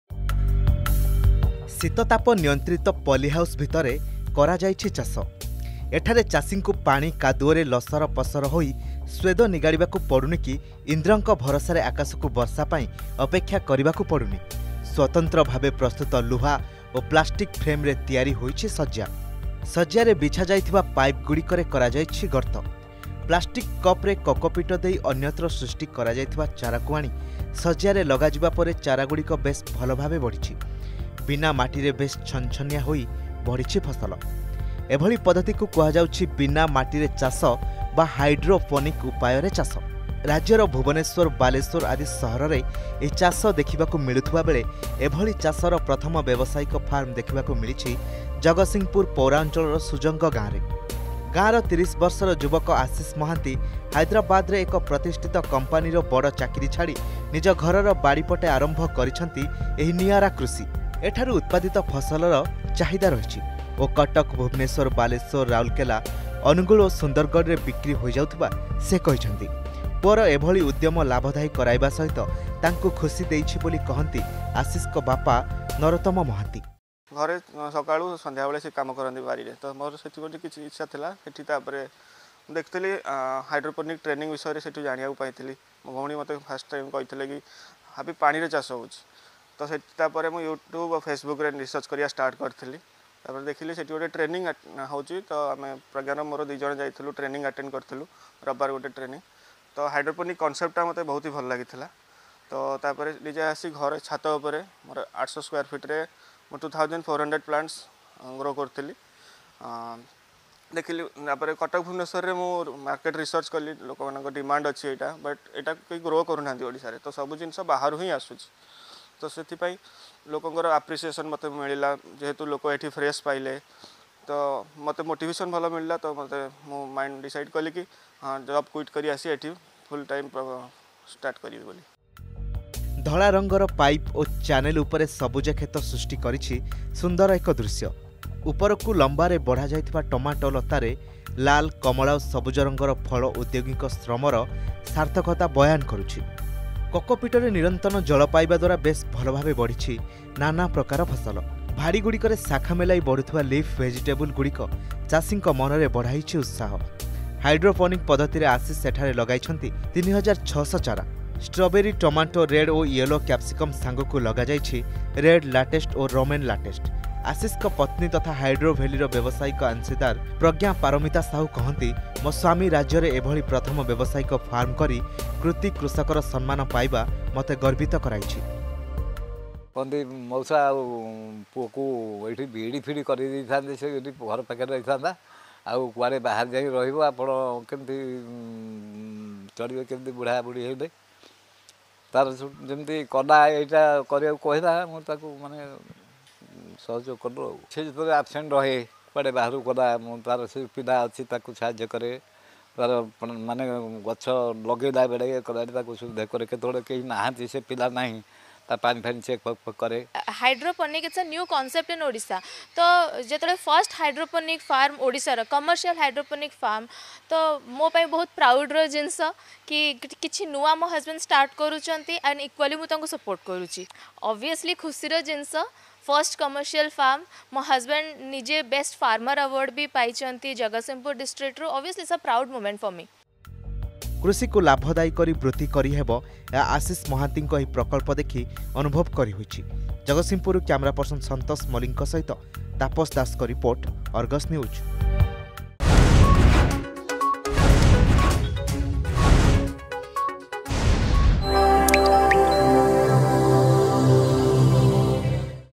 ରାଜ୍ୟର ପ୍ରଥମ ବ୍ୟବସାୟିକ ହାଇଡ୍ରୋପୋନିକ ଫାର୍ମିଙ୍ଗ
ହାଇଦ୍ରାବାଦରେ ପ୍ରତିଷ୍ଠିତ କମ୍ପାନୀର ବଡ଼ ଚାକିରୀ ଛାଡି ଜଗତସିଂପୁରର ୩୦ ବର୍ଷର ଯୁବକ ଆଶିଷ ମହାନ୍ତି ଯେତେବେଳେ ପ୍ରଥମ କରି ନିଜ ଘରର ଛାତ ଉପରେ ବିନା ମାଟିରେ କେବଳ ପାଣିରେ ବଢୁଥିବା ଫସଲ ଚାଷ କଲେ ଏହାର ଲାଭକୁ ନେଇ କିଏ କେତେ ପ୍ରକାର କଥା କହିଲେ । କିନ୍ତୁ ଆଶିଷ ନିରୁତ୍ସାହିତ ହୋଇନଥିଲେ । ବରଂ ଏହାର ସମ୍ପ୍ରସାରଣ ଓ ବଜାର ଉପରେ ଦୃଷ୍ଟି ଦେଇଥିଲେ ।  ଆଶିଷଙ୍କ ଏଭଳି ହାଇଡ୍ରୋପୋନିକ ପଦ୍ଧତିର ଚାଷ ରାଜ୍ୟର ପ୍ରଥମ ବ୍ୟବସାୟିକ ଫାର୍ମ ଭାବେ ପରିଚିତି ପାଇଥିବା ବେଳେ ଏଭଳି ଉଦ୍ୟମ ପାଇଁ ତାଙ୍କୁ ମିଳିଛି କୃତି କୃଷକର ସମ୍ମାନ